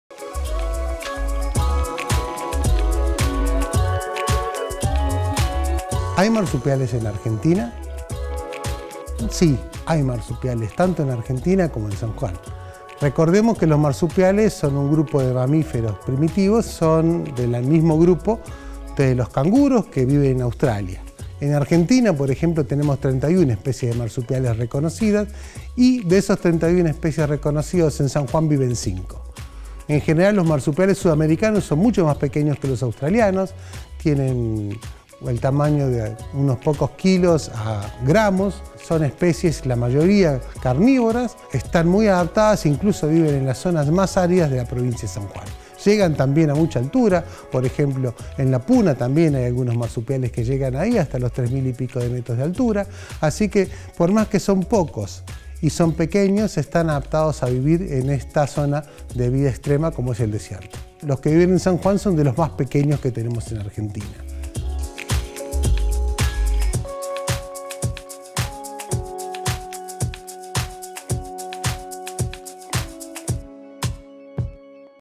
CIENCIA AL PASO - ¿Hay marsupiales en la Argentina?
Responde Carlos Borghi, biólogo, docente e investigador de la Facultad de Ciencias Exactas, Físicas y Naturales de la UNSJ.